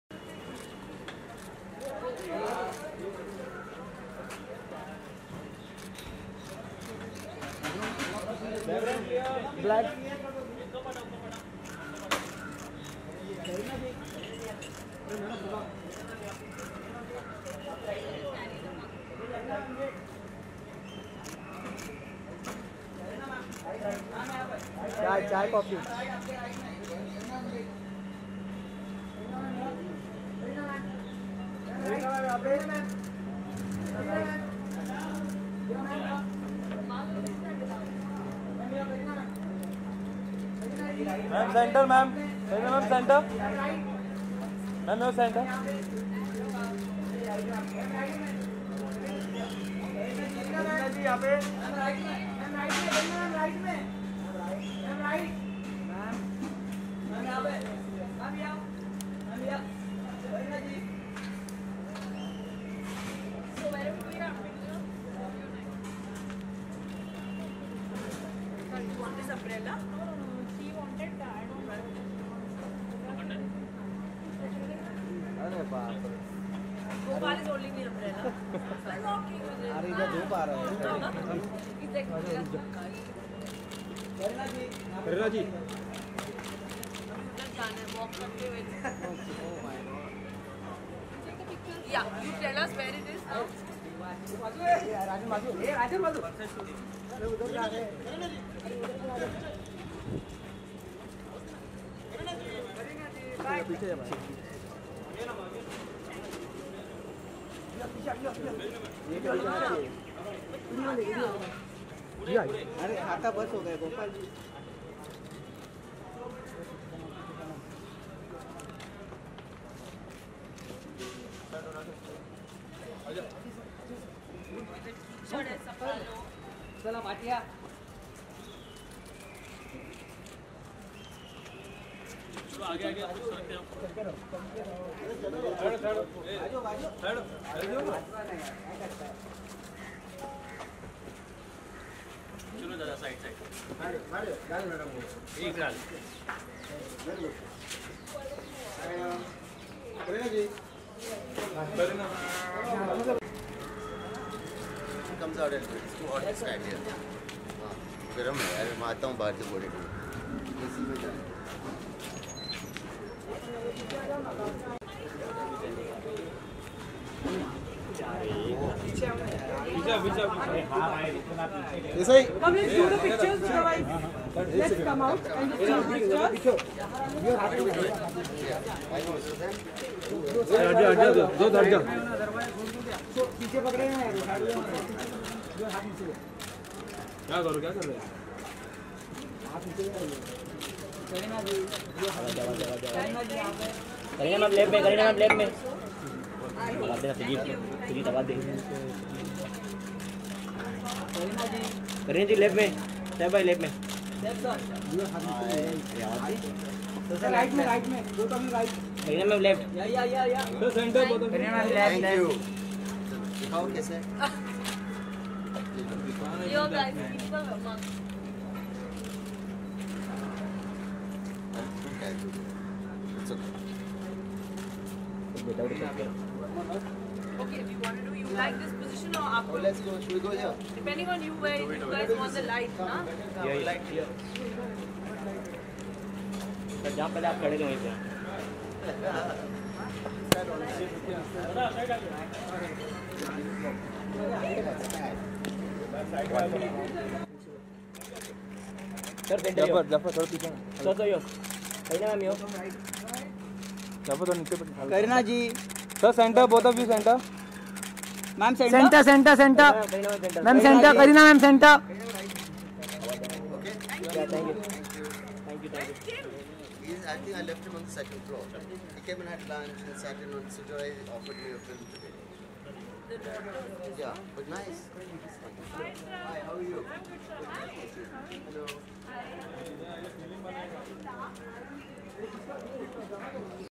Kareena Kapoor with Saif Ali Khan Looks Cool Together at Studio | Boldsky
Kareena Kapoor Khan and Saif Ali Khan pose together Romantically at Mumbai studio. The couple were spotted at a studio in Mumbai today and they even posed for the shutterbugs present out there together which has now made us wonder what cooking. Watch the video to know more about this !